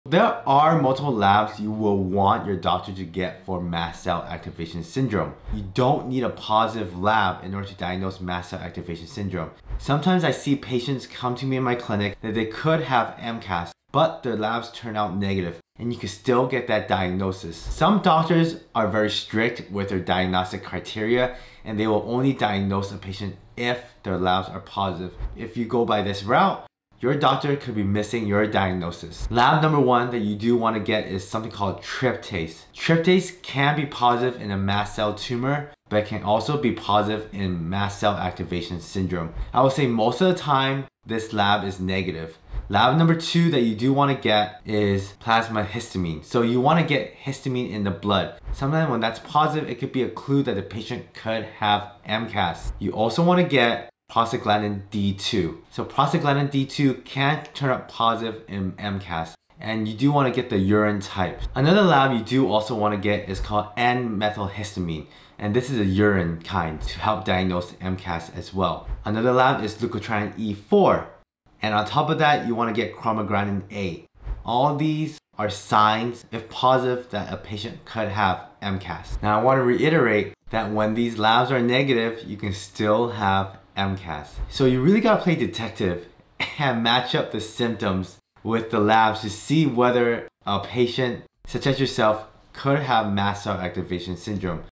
Important Labs For MCAS (Mast Cell Activation Syndrome)
Struggling with unexplained symptoms and suspect Mast Cell Activation Syndrome (MCAS)? Learn about the most important labs that can help support an MCAS diagnosis — including tryptase, plasma histamine, prostaglandin D2, N-methylhistamine, leukotriene E4, and chromogranin A.

Testing the right markers can bring clarity when symptoms are confusing or overlooked. If you're searching for answers, these labs could be a valuable step forward.

where I send out information on news on rheumatology, autoimmune disease, and integrative medicine!

My clinic is located in Newport Beach, California. I see patients virtually throughout USA and worldwide.